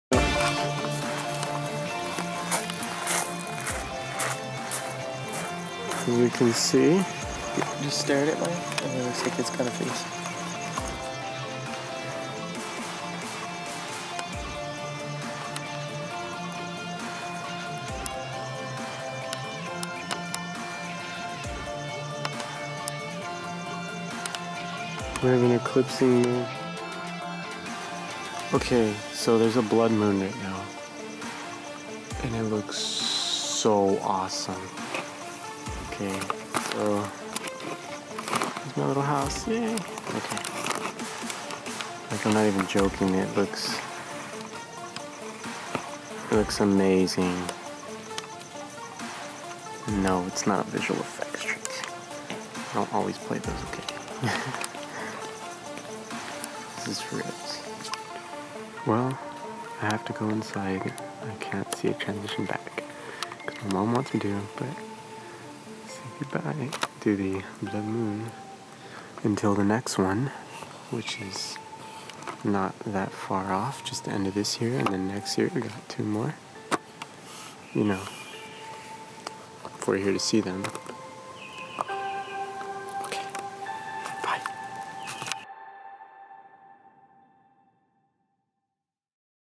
April 15 2014 Blood Moon (Arizona)
A simple (horribly edited) but for fun highlight of the blood moon this April 15, 2014! Stayed out there for 4 hours and enjoyed every second :)! Not every day this happens